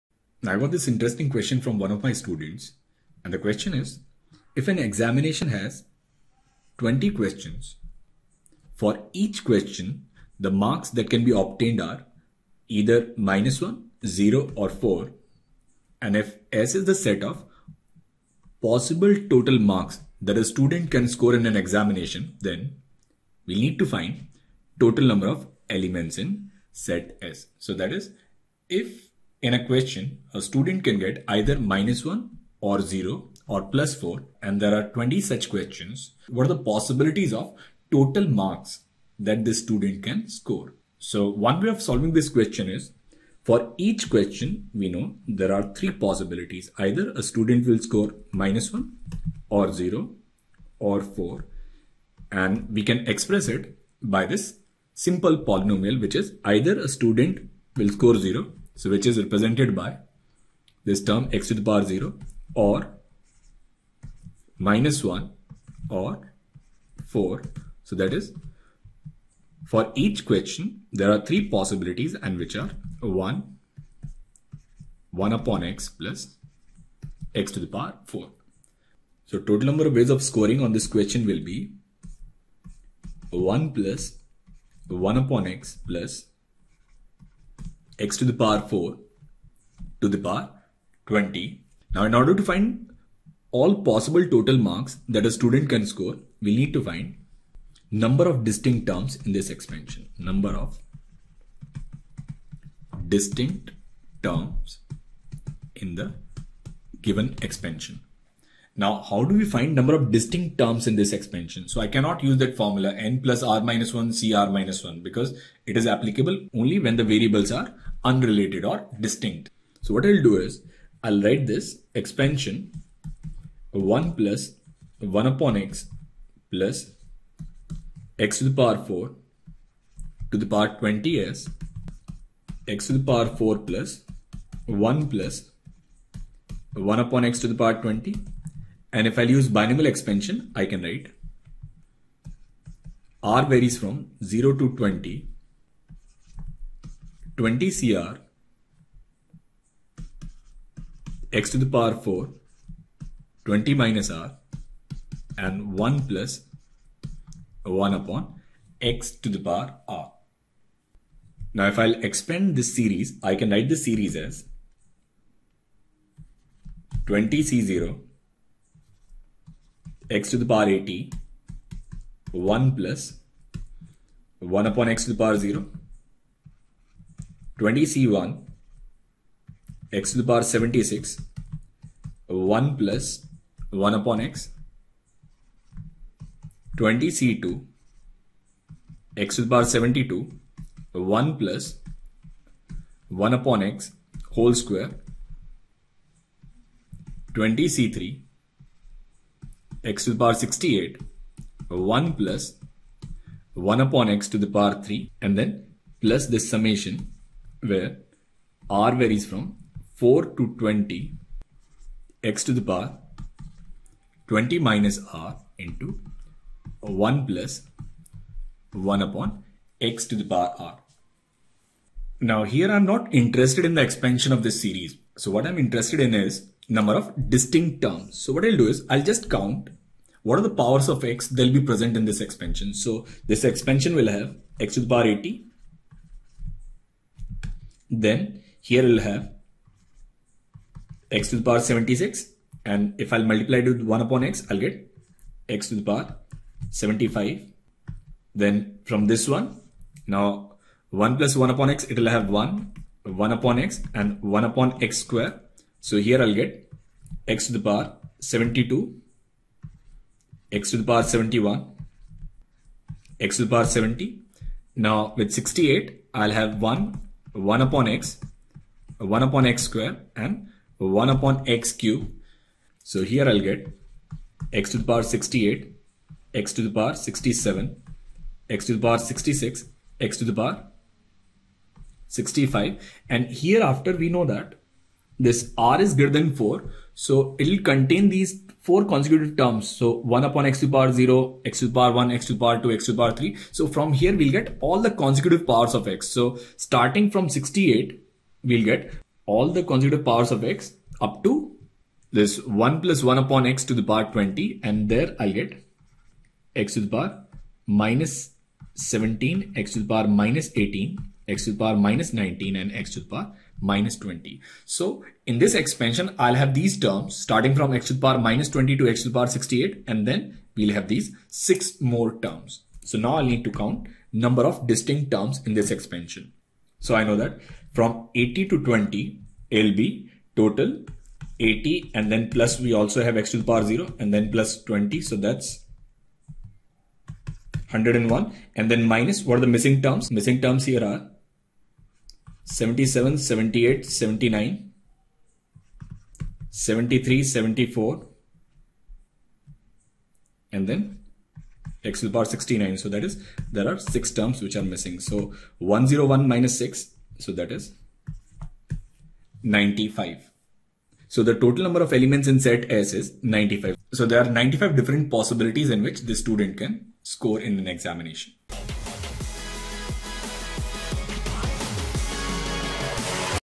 Math Marvels: Possible score from 20 questions when -1, 0 or 4 marks is obtained for each question
Indian Statistical Institute (ISI) B Stat exam question: Math Marvels: An examination has 20 questions. For each question the marks that can be obtained are either -1, 0 or 4. Let S be the set of possible total marks that a student can score in the examinaion.